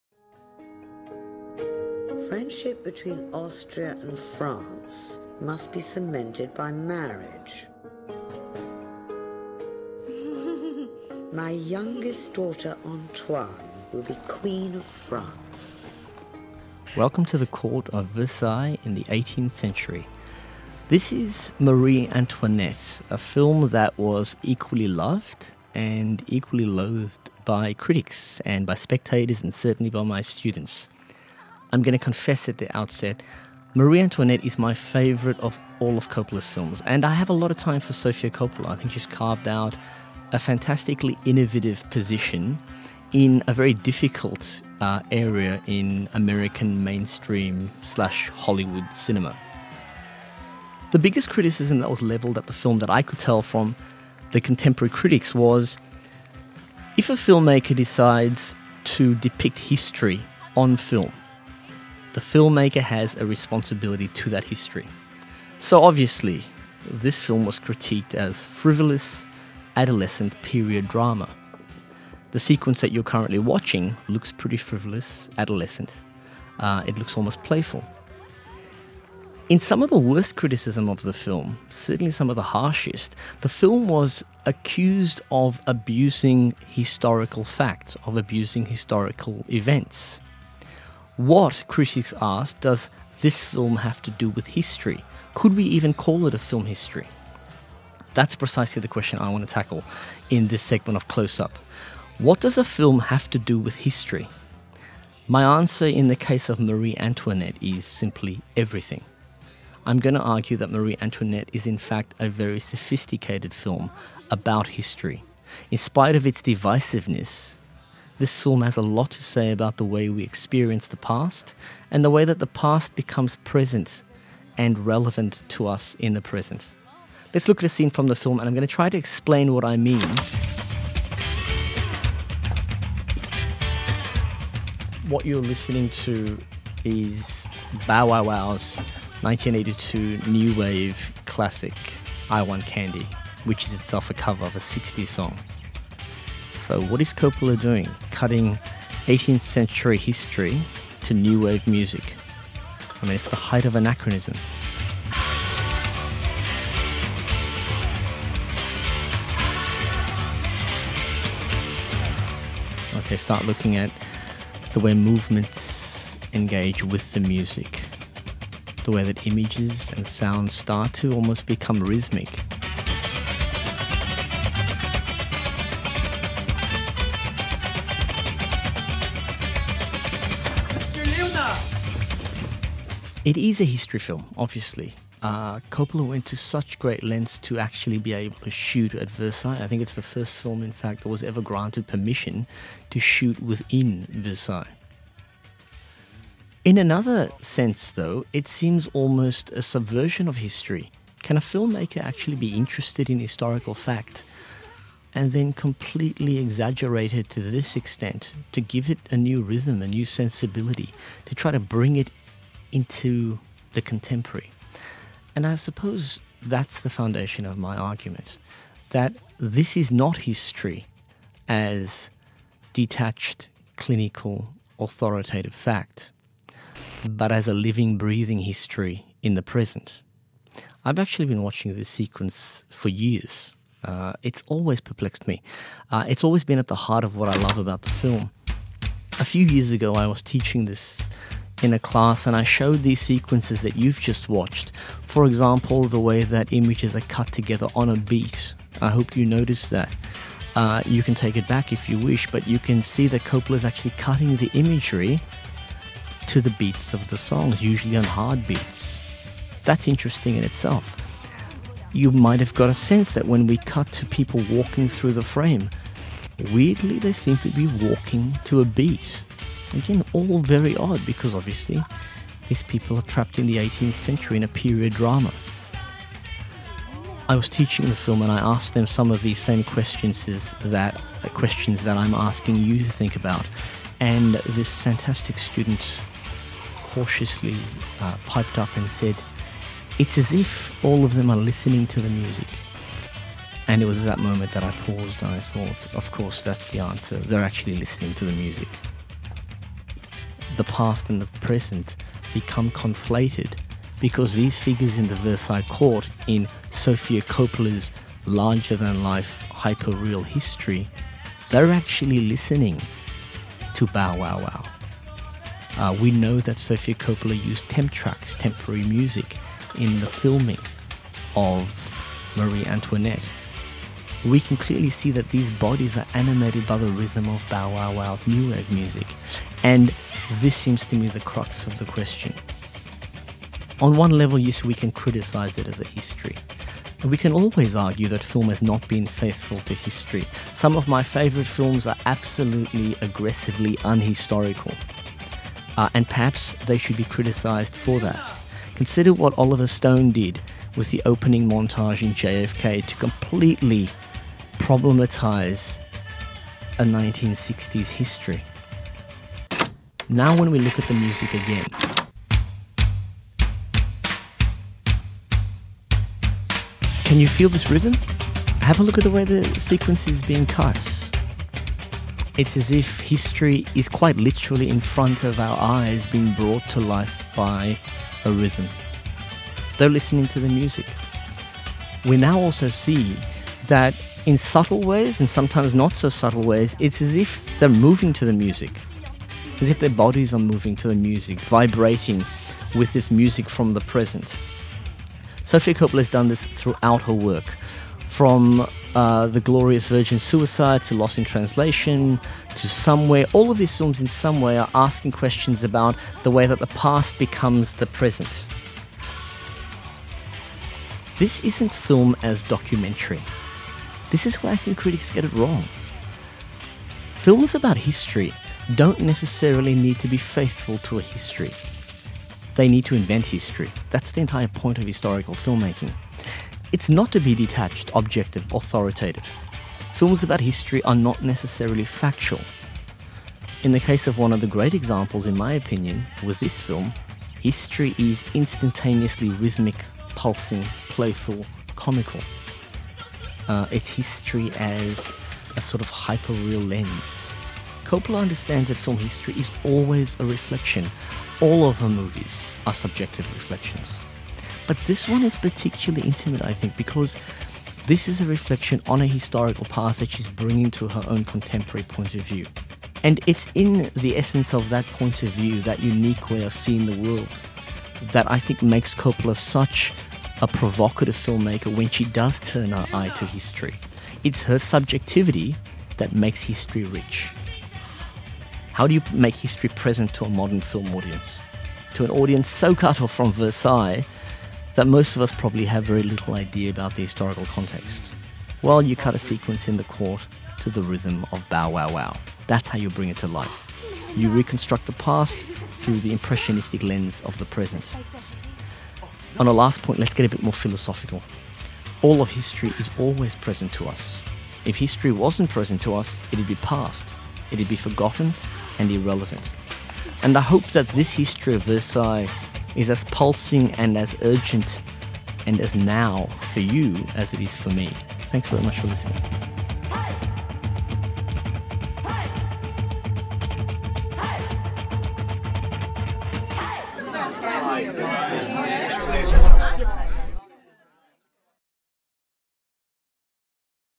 Close-Up: Marie Antoinette, 2006
Sofia Coppola’s 2006 film Marie Antoinette divided both critics and audience alike with its playful depiction of historical events. Many critics felt that by choosing to depict history on film, Coppola had a responsibility to be faithful to that history.

While some of the harshest critics accused the film of abusing historical facts, Coppola’s Marie Antoinette is a sophisticated recreation of the past through the impressionistic lens of the present. The film has a lot to say about how we experience the past, and how that past is relevant to us today.